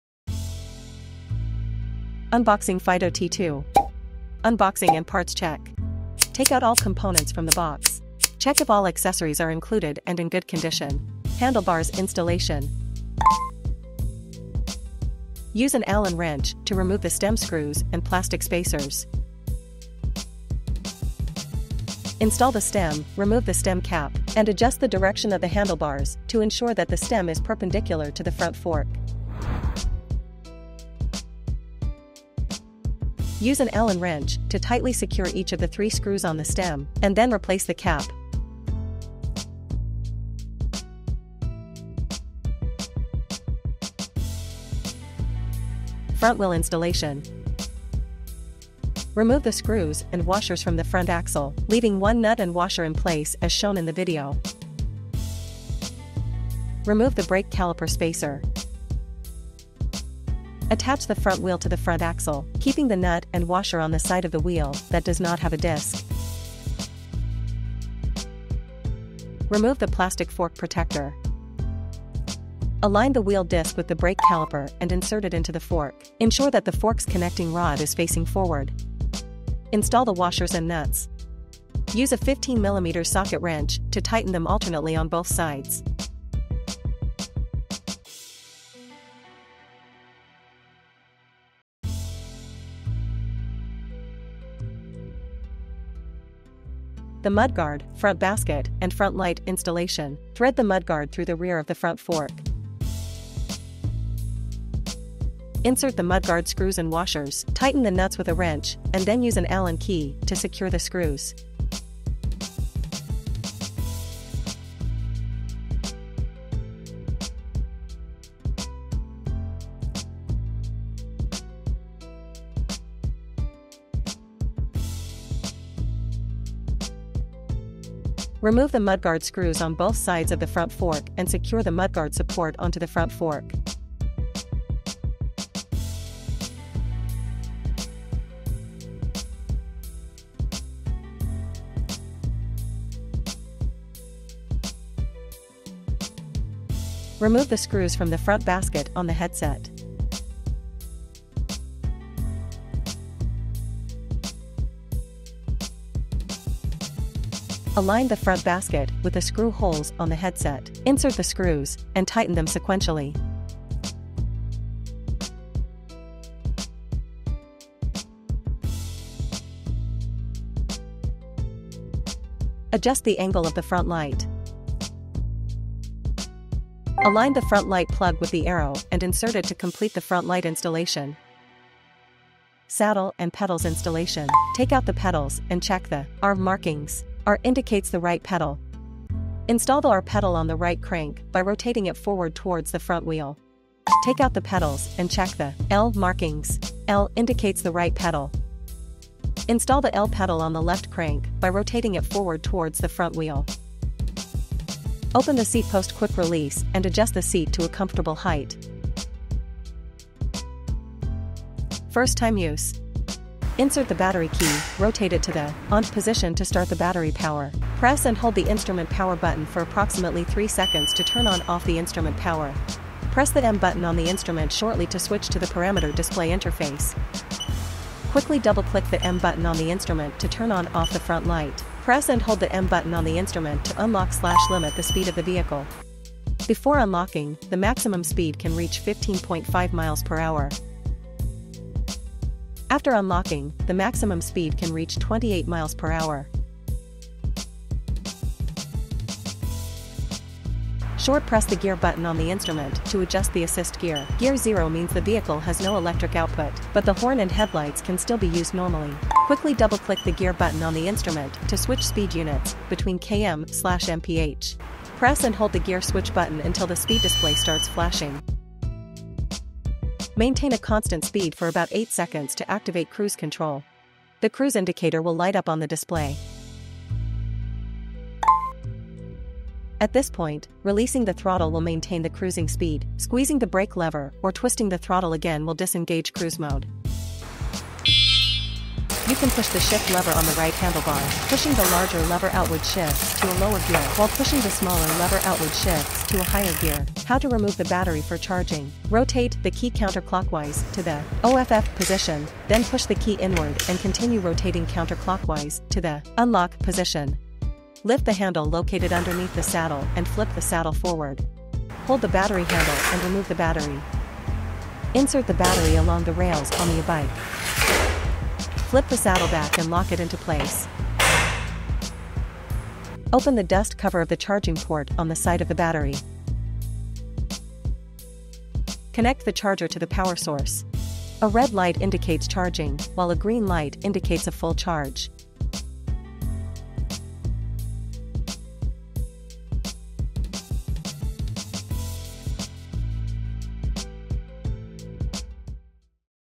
Fiido T2 unboxing video
Explore the comprehensive unboxing and assembly of the Fiido T2 in this informative video. We'll introduce you to all the included accessories, guide you through the installation of the handlebars and front wheel, demonstrate how to set up the mudguard, front basket, and front light, and show you how to install the saddle and pedals for optimal comfort. Join us as we embark on the first ride with the Fiido T2 and learn how to easily remove the battery for charging, ensuring a seamless and enjoyable riding experience.

Key Features:

✅ Ultra-long range of 110km with a 998.4Wh battery capacity
✅ Robust 750W rear motor providing ample torque
✅ Alloy front suspension and one-piece-wheel fat-tire for a smooth ride
✅ 4-piston hydraulic brake system and stable full-support kickstand
✅ Maximum payload capacity of 200kg and over 12 accessory choices

Additional Highlights:

✅Shimano 7-speed gearing
✅Max speed of 45km/h
✅App-controllable throttle activation
✅Ultra-bright headlights with turn-signal capability when paired with accessories
✅5 PAS (Pedal Assist System) modes
✅Intelligent LCD display
✅App connectivity for remote control and monitoring
✅Fiido Mate smartwatch integration for enhanced connectivity
✅Compatibility of T2 battery with Fiido T1 Pro ebike
Join us as we explore the Fiido T2's extensive features and see how it can elevate your daily transportation needs.